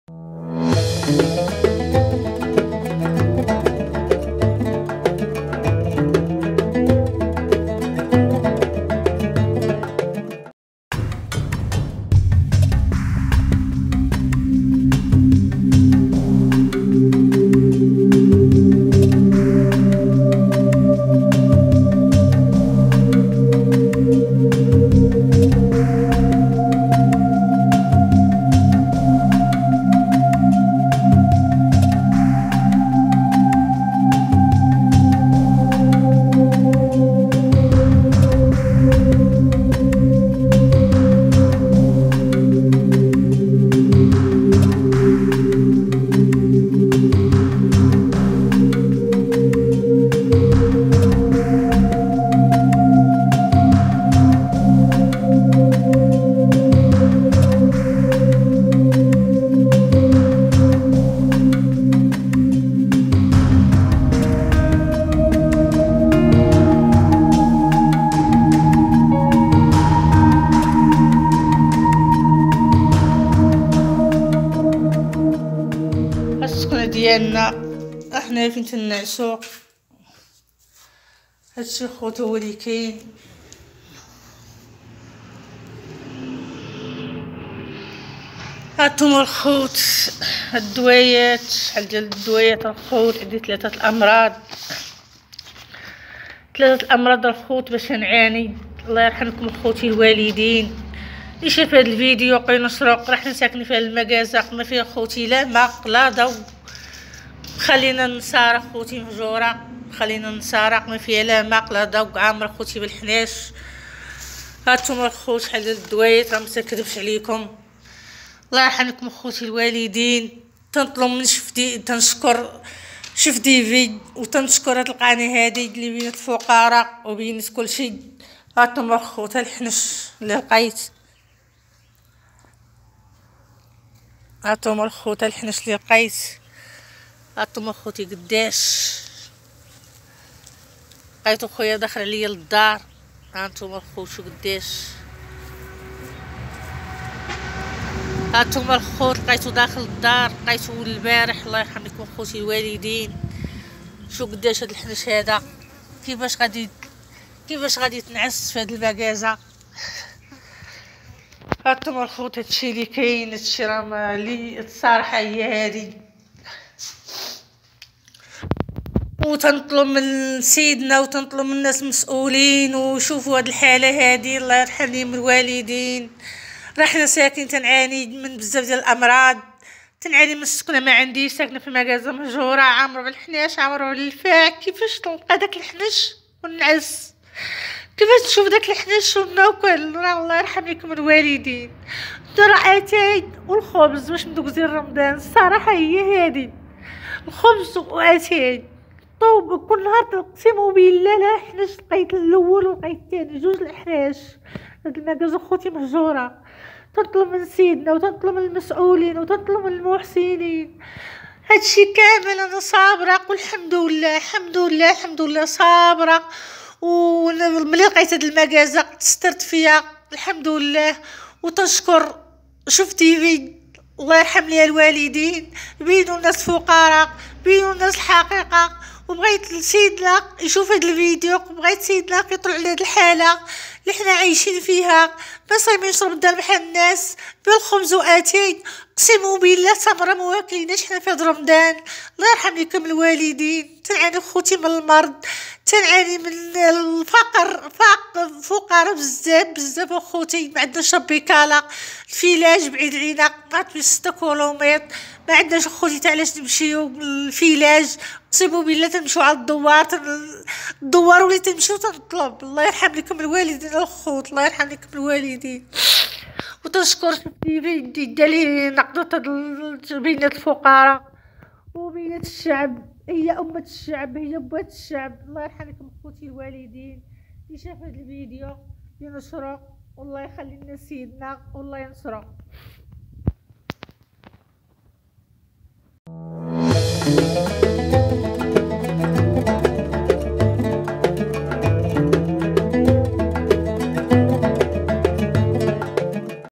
في 2020..سيدة كتعيش فخطر بسبب اقتحام الأفاعي لمنزلها الغريب والصادم بقلب بني ملال
رصدت شوف تيفي الحالة المأساوية لأسرة تعيش بمنزل لا تتوفر فيه أبسط مقومات العيش.